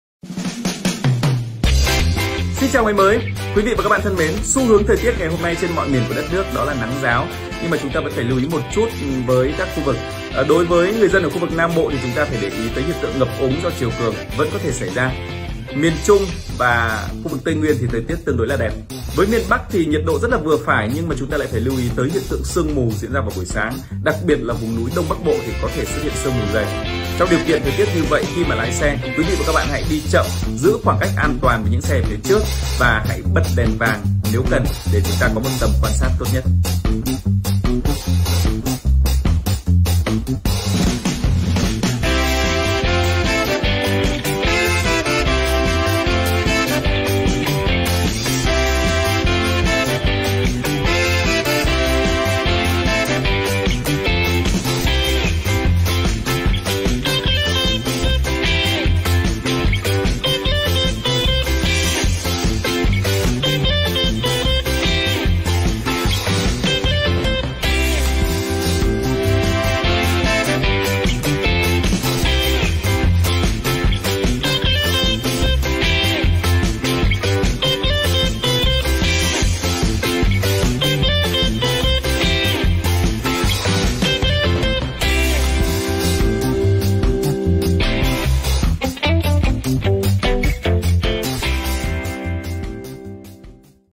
Thời tiết chào ngày mới 09/11/2022: Cả nước xu hướng tạnh ráo, có nắng | VTVWDB
Triều cường đánh sập nhiều nhà dân ở Quảng Ngãi